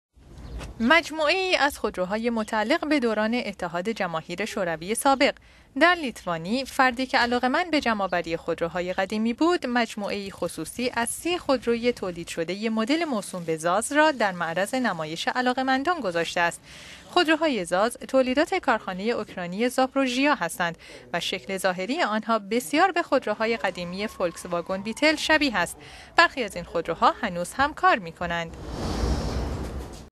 نمایش خودروهای متعلق به دوران اتحاد جماهیر شوروی سابق
*خبر کوتاه*

در لیتوانی، فردی به نام آندریوس چیکوتاس که علاقمند به جمع آوری خودروهای قدیمی بود، مجموعه ای خصوصی از ۳۰ خودروی تولید شدۀ مدل موسوم به «ZAZ Zaporozhets» را در معرض نمایش علاقمندان گذاشت.

خودروهای «ZAZ» تولیدات کارخانه اوکراینی زاپروژیا (Zaporojie) هستند و شکل ظاهری آنها بسیار به خودروهای قدیمی فولکس واگن بیتل شبیه است.

برخی از خودروهای این مجموعه هنوز هم کار می کنند.…